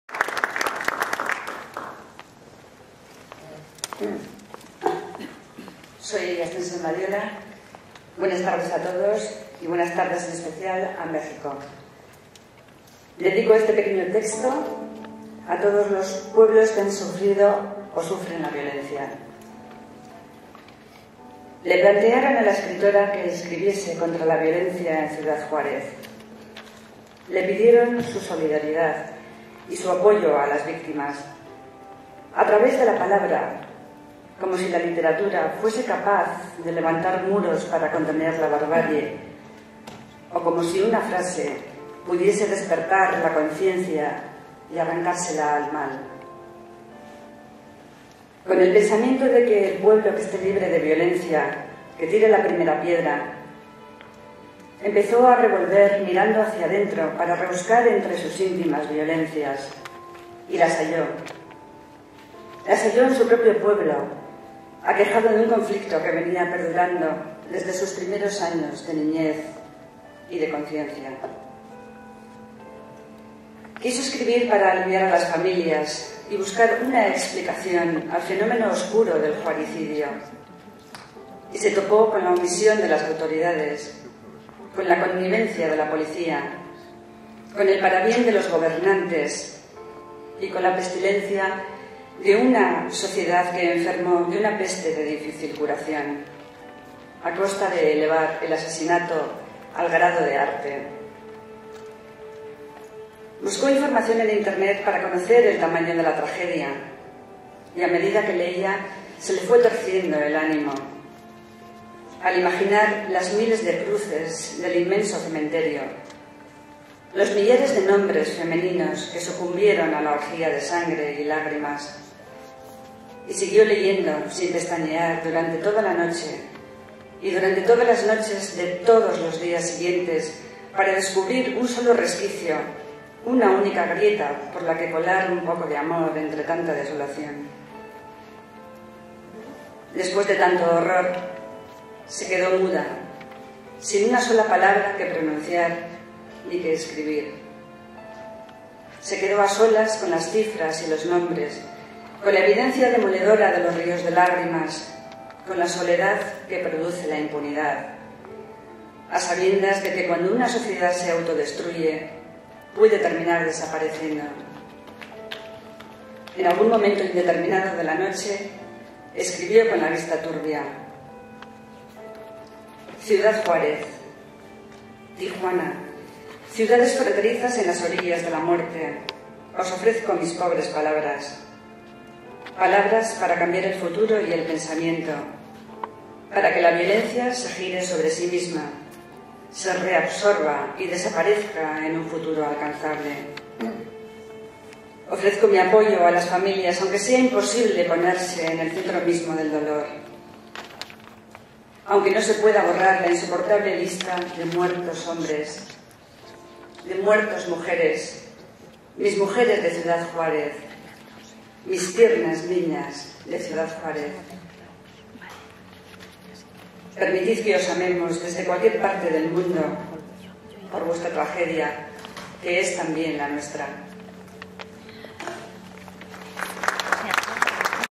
Escritores por Ciudad Juarez en Bilbao - Ascensión Badiola
En septiembre de 2011 un grupo de jóvenes activistas, escritoras y escritores, decidieron salir a la calle, bajo el lema "Escritores por Ciudad Juárez" desafiando a las organizaciones criminales y a quienes las encubren, para manifestarse abiertamente y a cara descubierta y reclamar para el pueblo de Juárez, la paz y la recuperación de los espacios públicos, en favor de la convivencia y la libertad.

El 1 DE SEPTIEMBRE DE 2012, hemos celebrado en numerosos pueblos y ciudades del planeta, las lecturas solidarias "ESCRITORES POR CIUDAD JUÁREZ". Estas lecturas están convocadas en solidaridad con Ciudad Juárez, en representación de todo el pueblo México y por extensión de cualquier otro rincón del planeta donde el miedo, consecuencia última de la violencia, es utilizado para imponer la voluntad y los intereses de los grupos de poder sobre los derechos y la dignidad de los pueblos y sus gentes.

En nombre del colectivo Escritores por Ciudad Juárez lanzamos esta llamada a la solidaridad y la movilización. Han respondido 144 poblaciones, en 25 países y 4 continentes, que hemos arropado el 1 de septiembre, a las y los jóvenes de Ciudad Juárez, y hemos provocado que cada recital sea un luz encendida contra la violencia y por la paz, ya sea en grandes capitales como en pueblos perdidos.